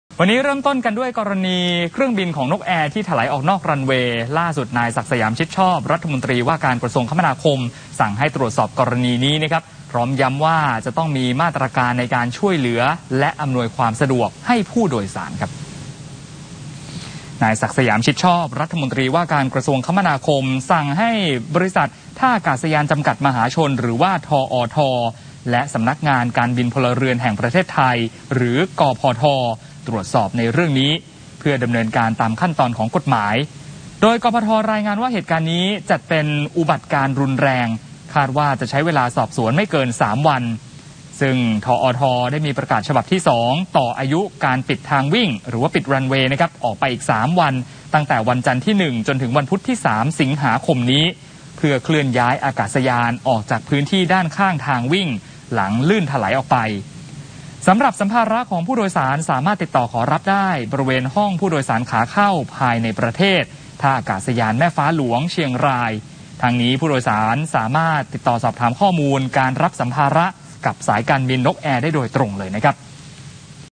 "ศักดิ์สยาม" สั่งเร่งสอบ นกแอร์ ไถลออกรันเวย์ lข่าวค่ำNIGHTLY NEWSl 31 ก.ค.65
"ศักดิ์สยาม" สั่ง 2 หน่วยงาน เร่งตรวจสอบ นกแอร์ไถลออกนอกรันเวย์ที่สนามบินแม่ฟ้าหลวง เหตุที่เกิดขึ้นจัดเป็นอุบัติการณ์รุนแรง กำชับต้องมีมาตรการช่วยและอำนวยความสะดวกผู้โดยสาร